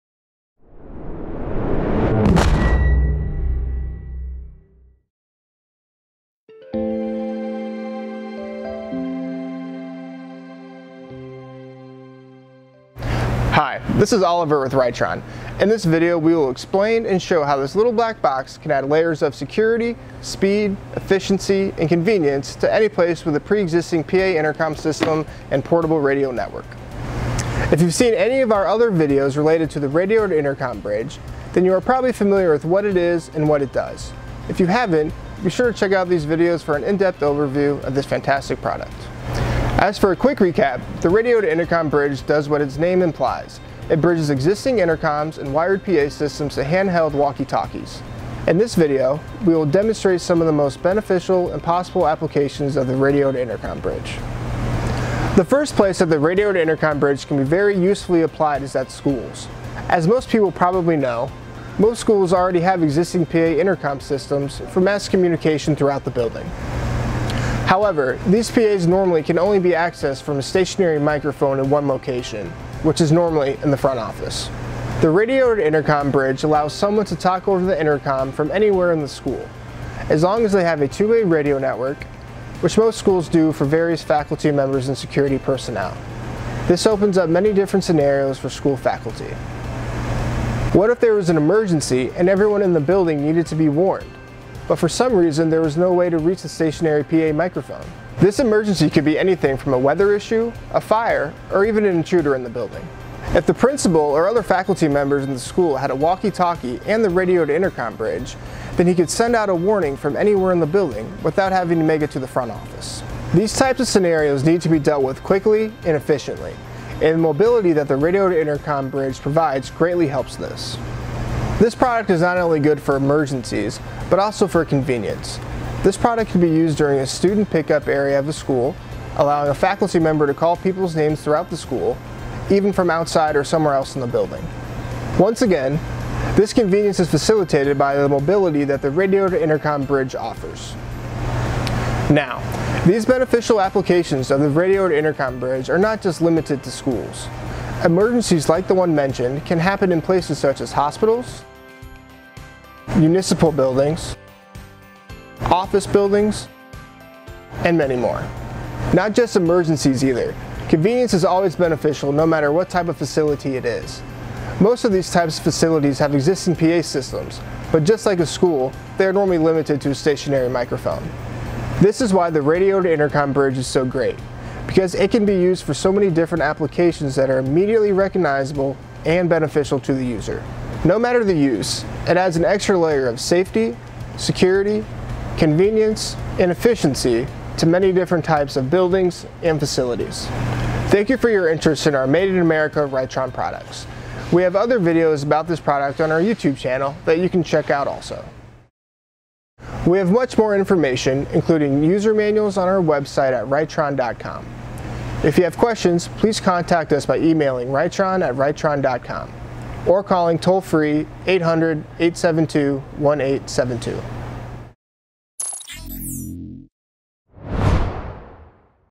Application Video - Radio-To-Intercom Bridge™
This video explains and shows some of the many different possible and beneficial applications of the Ritron Radio-To-Intercom Bridge™.

Video produced by Matt Voss